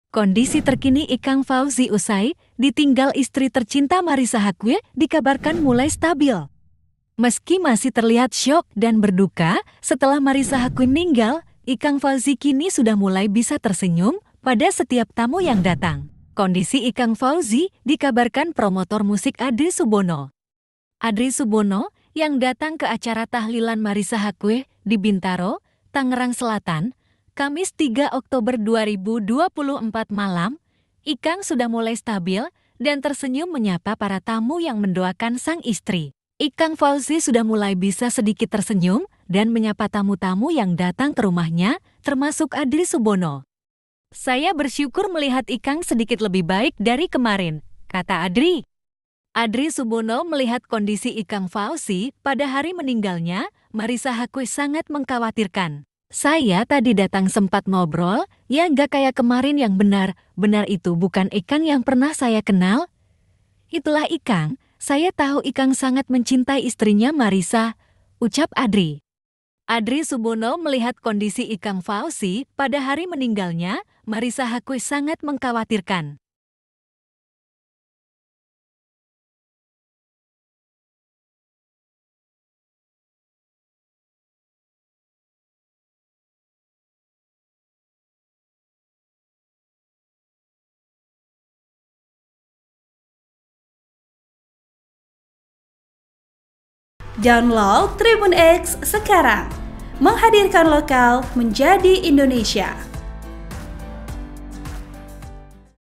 KONDISI TERKINI IKANG FAWZI USAI MARISSA HAQUE MENINGGAL, SUDAH BISA TERSENYUM DI TAHLILAN ISTRI
Adrie Subono melihat kondisi Ikang Fawzi pada hari meninggalnya Marissa Haque sangat mengkhawatirkan.

Adrie Subono bahkan seperti tak melihat Ikang Fawzi yang dikenalnya pada hari pertama.

"Saya tadi datang sempat ngobrol ya gak kayak kemarin yang benar-benar itu bukan Ikang yang pernah saya kenal. Itulah Ikang, saya tahu Ikang sangat mencintai istrinya Marissa," ucap Adrie.

ve: elisabet.horman
vu: elisabet.horman